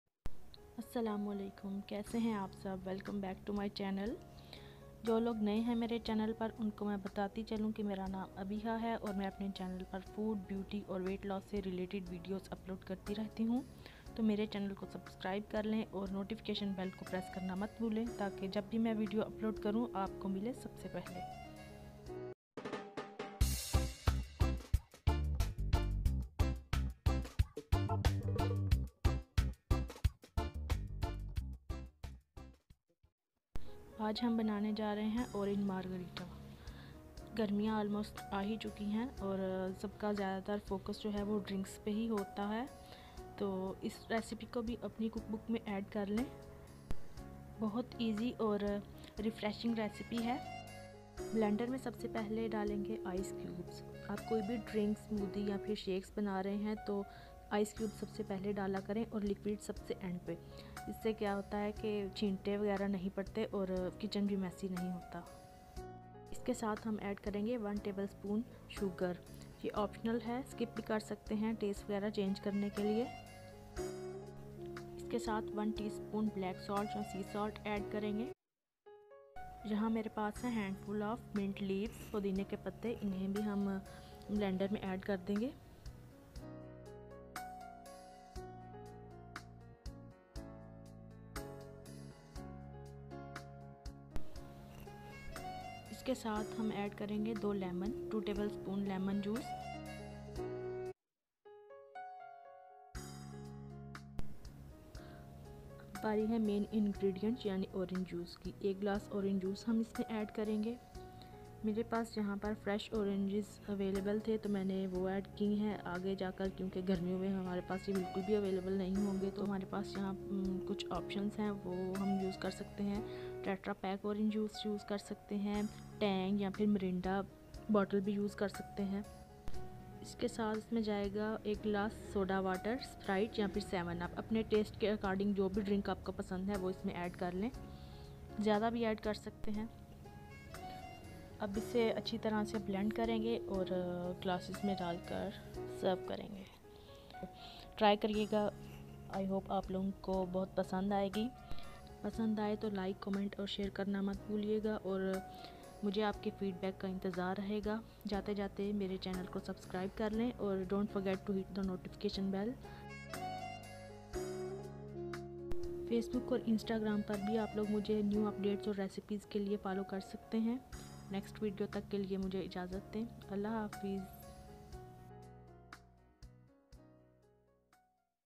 Mint Orange Lemonade
Ingredients:
1 glass lemon juice (fresh/ tang/ mrinda)
1 glass soda water (sprite/ 7up)
2 lemons
Hand full of mint leaves
1 tbsp sugar (optional)
1 tsp black salt
Ice cubes